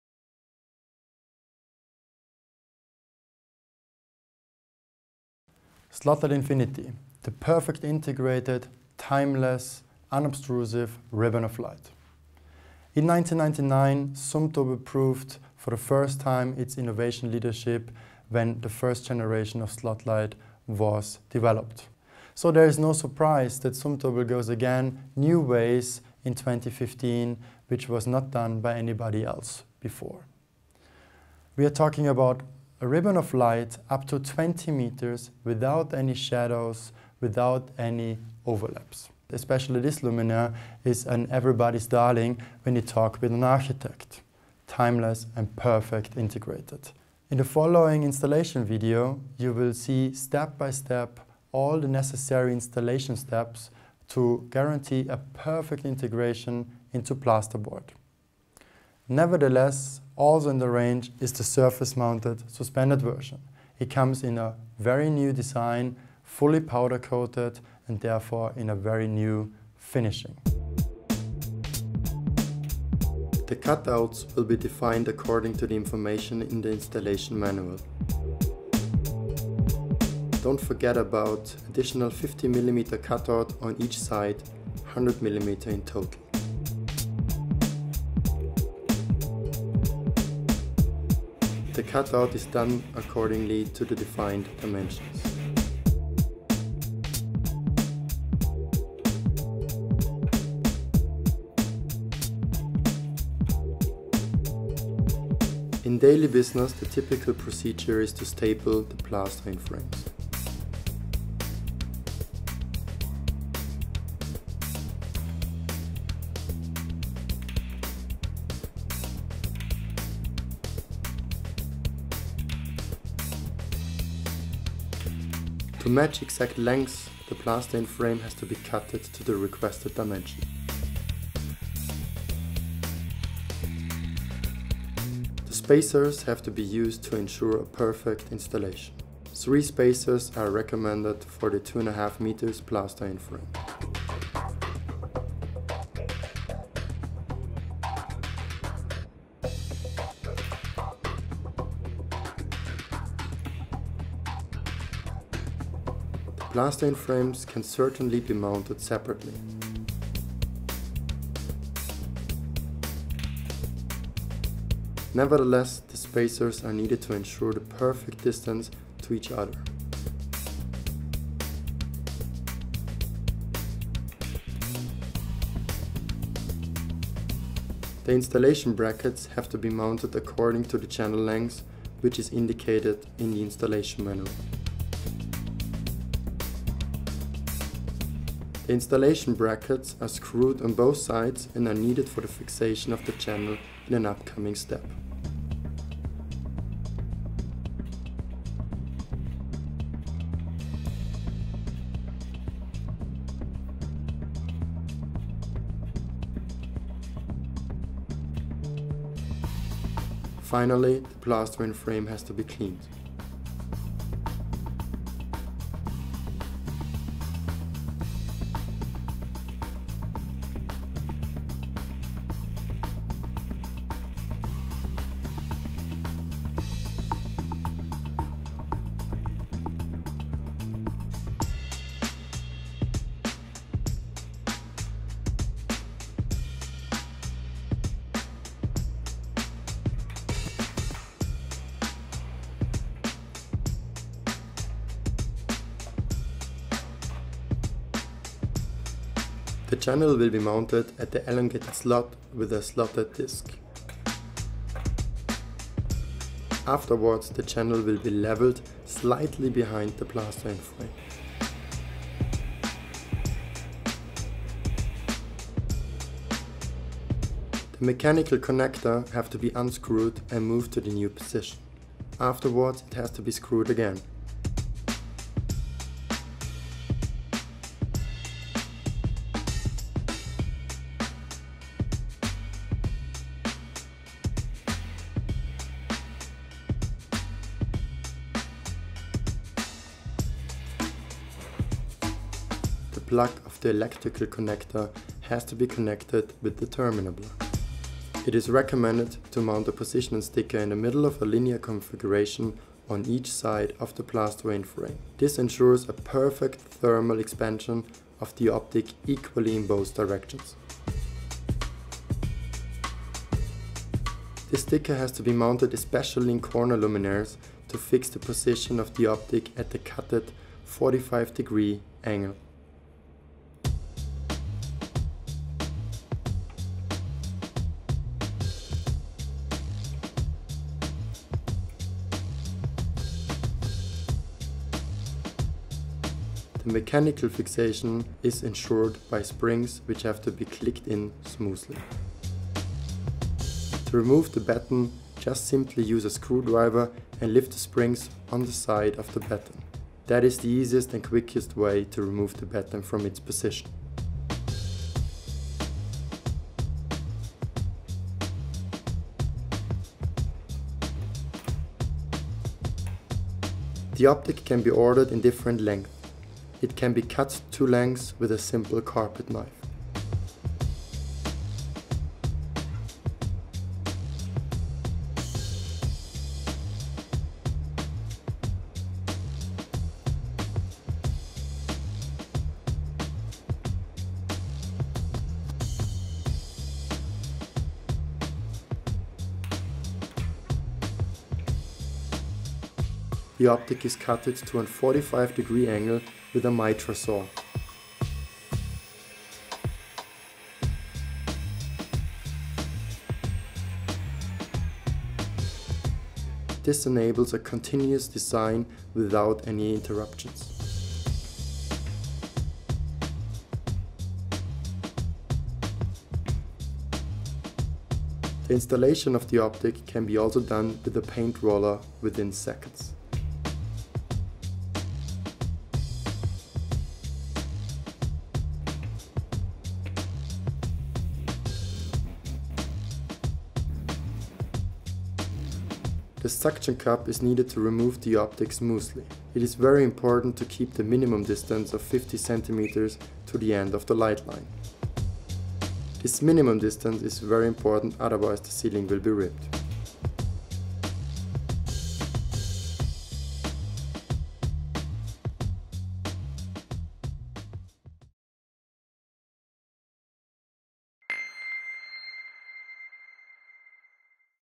Zumtobel Installation Tutorial - SLOTLIGHT infinity (English)
With SLOTLIGHT infinity product range, Zumtobel combines pure feel-good light with excellent lighting effects in one LED light line. Light is not only directed along homogeneously lit lines, but also skilfully guided around corners. The successful solution is now being extended by intelligent innovations which considerably facilitate handling and lighting design: with SLOTLIGHT infinity, Zumtobel presents this spring the world’s first consistent LED light line – opening up a new dimension in design.